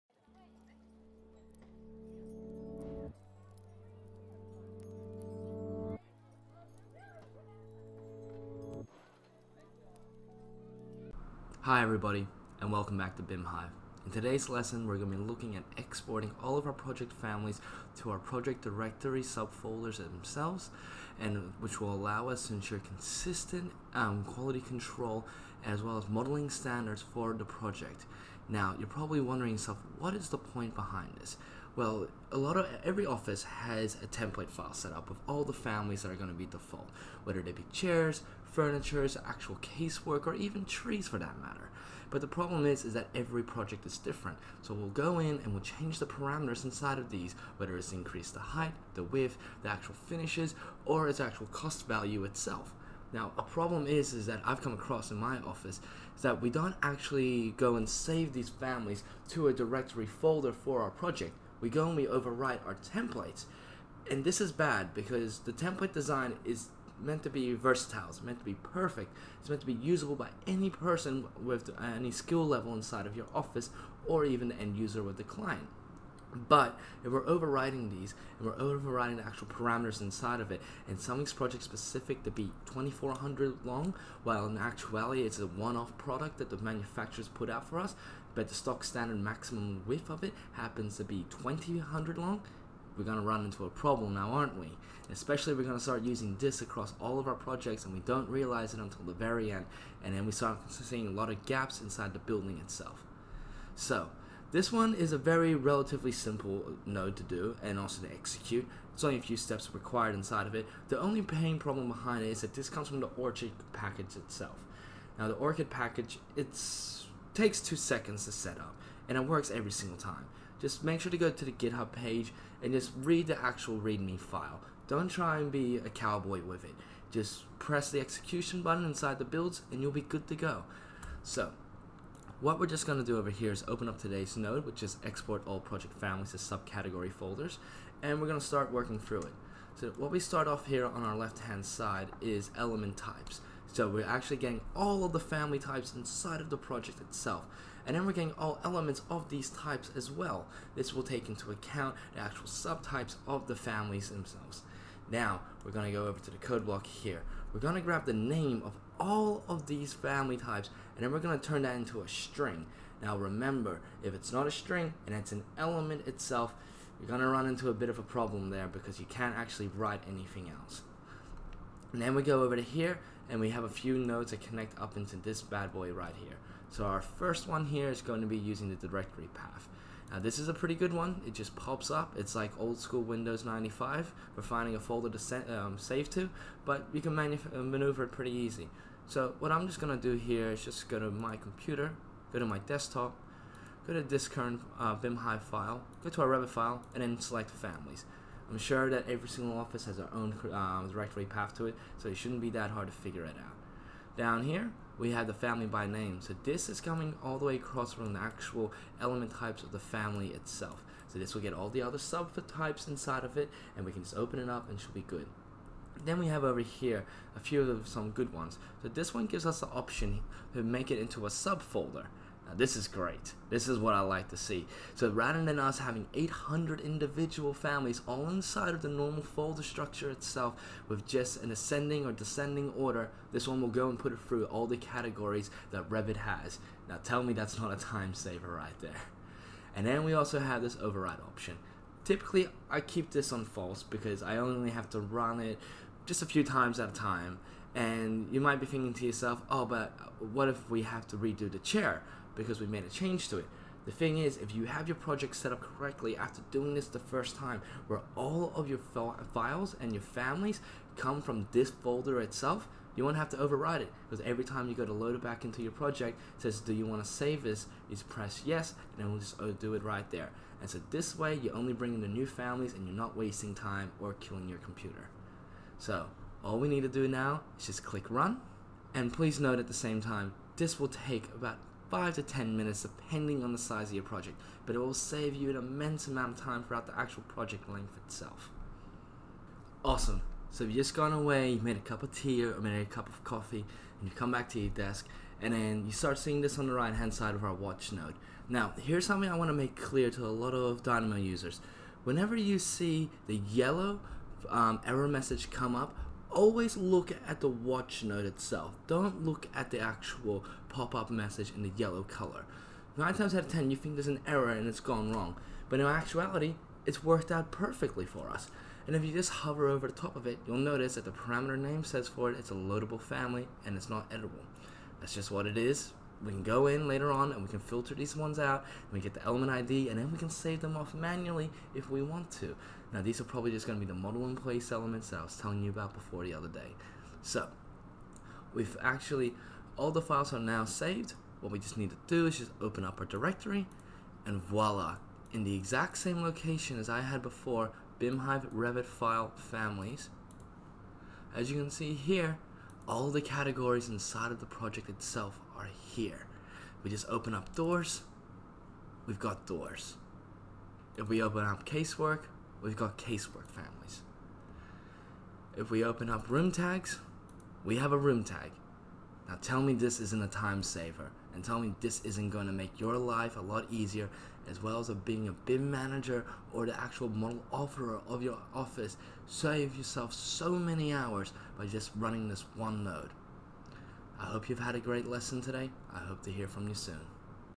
Dynamo tutorials - Episode 9 - Exporting your project families to your project family directory
Welcome back to our on going Dynamo tutorial series.

In today's episode we are going over how to export your project families to your project family directory.

We will be going over the purpose of this script and explaining the significance it makes to your office family standards and how it allows for intermediate model QA to help improve the model health quality.

Current builds and packages
Dynamo 1.3.2